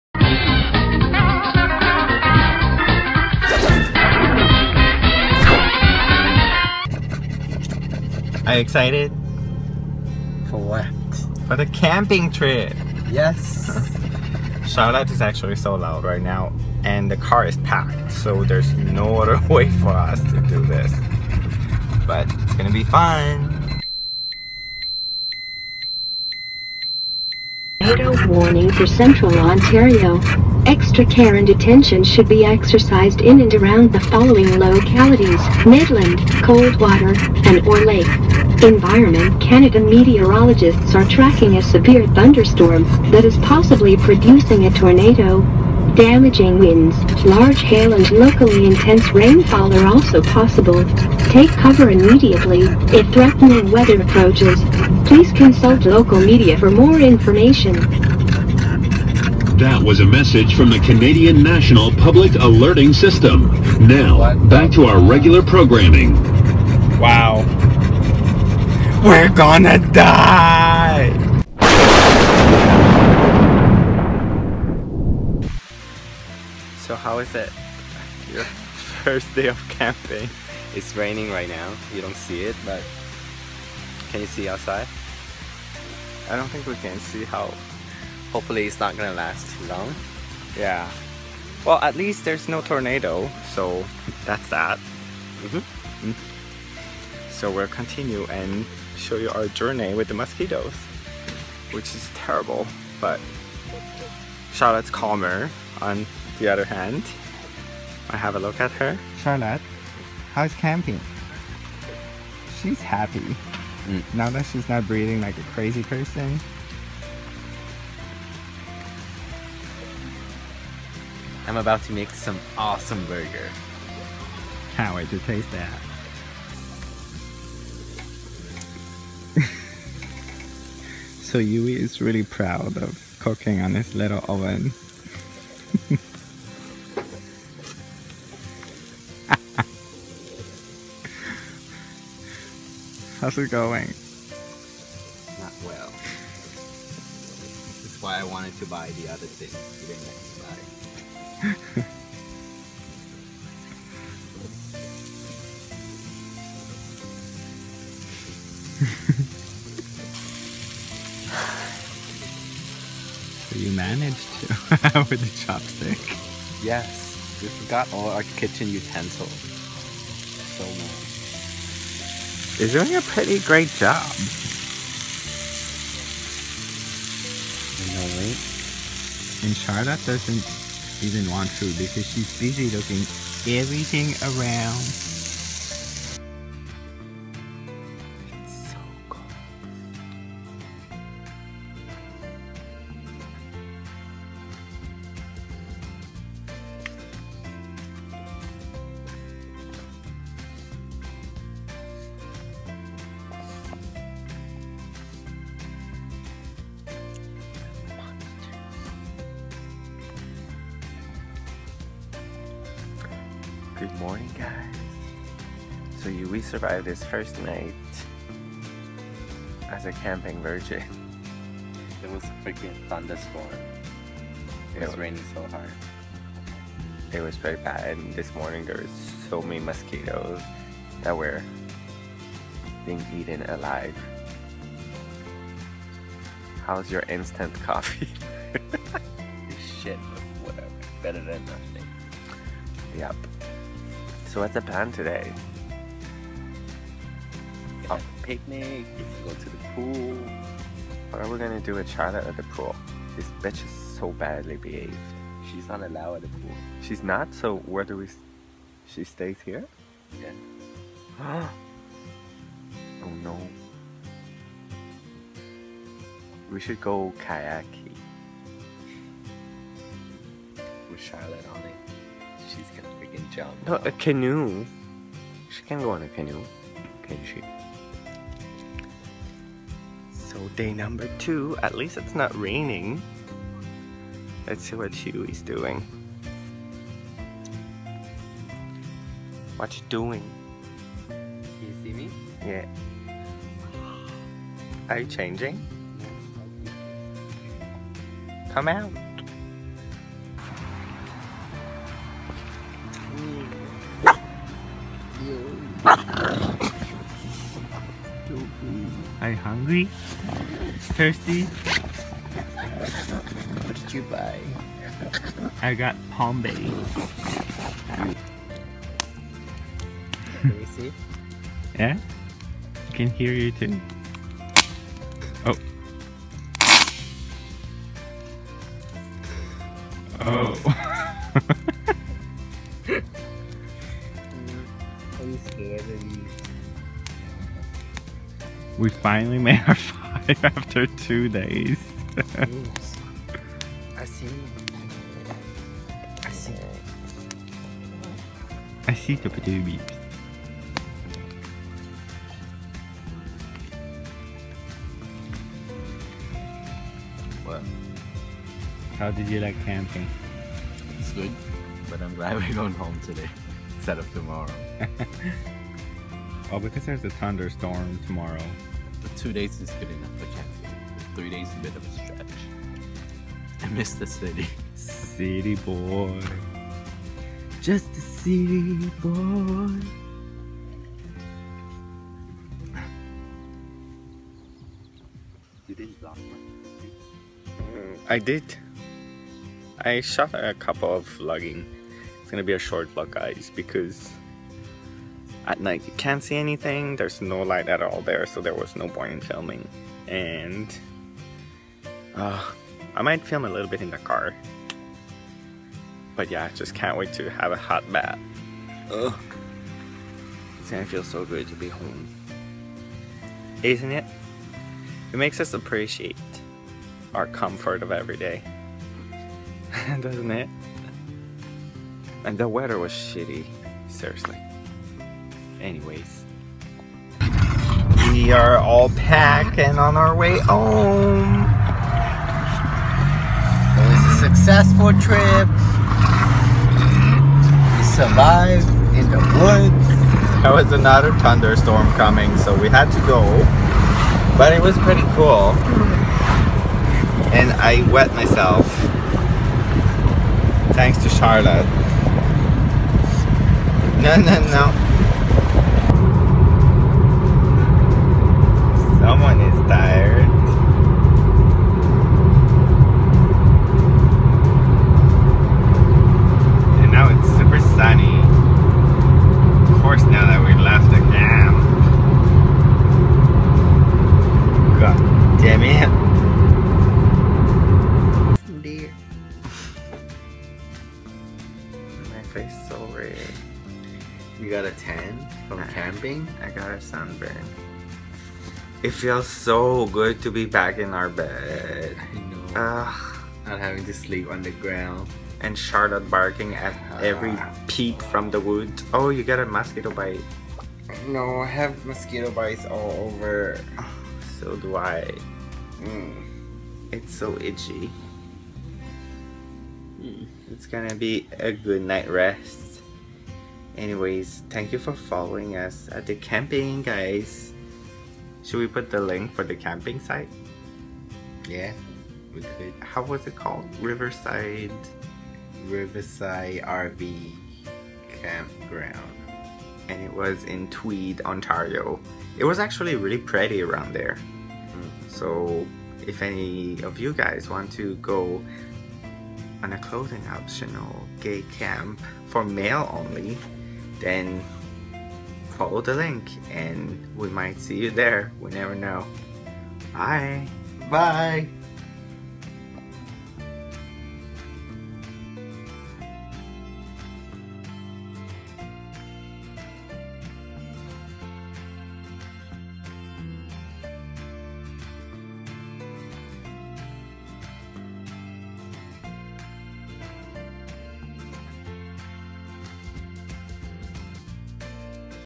Camping vlog in a tornado !!!
Huey and David vlog on a camping trip and the weather is not playing nice! Will they make it through the trip? Come visit an amazing clothing optional camping site exclusively reserved for gay men!